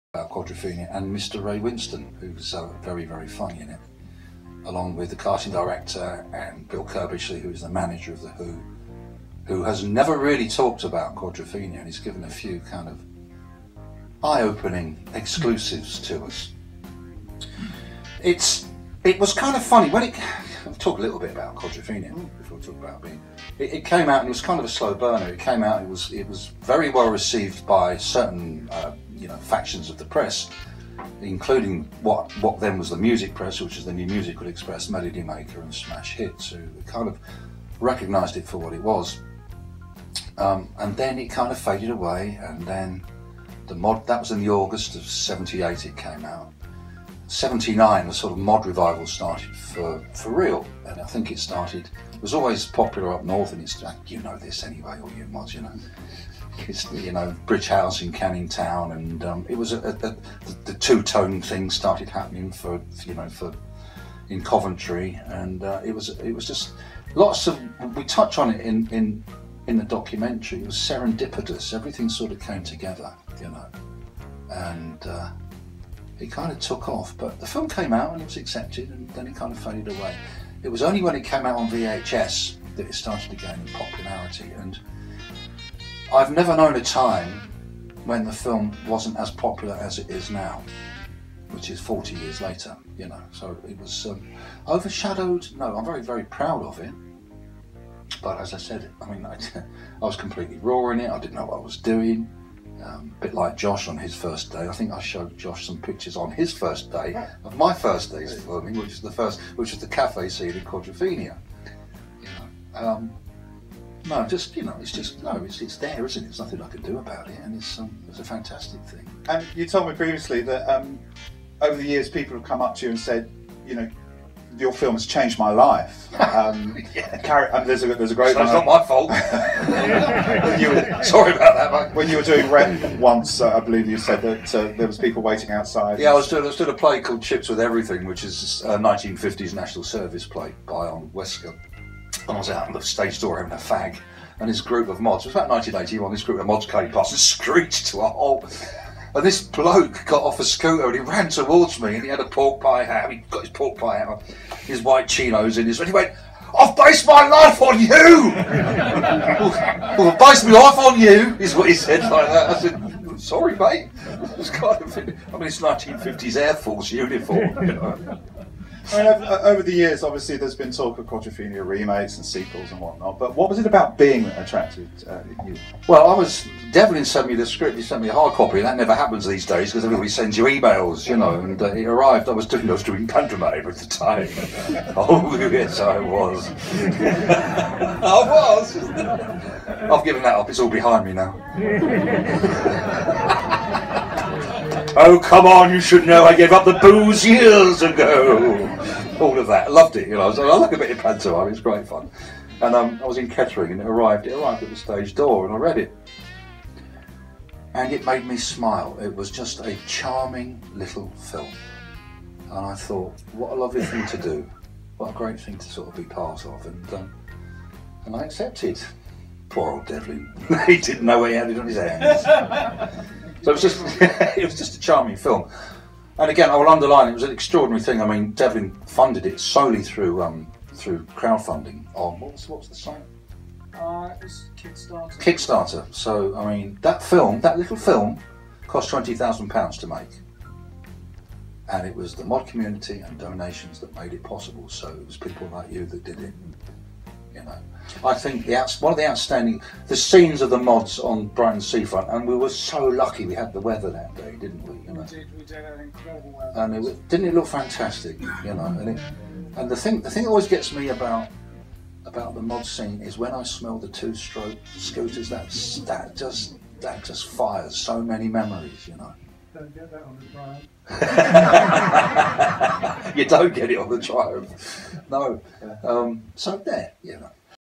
(Being) Devlin Crow ,with simon wells and Mark wingett..
Being film premier Lewes  Devlin crow ,Simon wells  Mark wingett (Dave)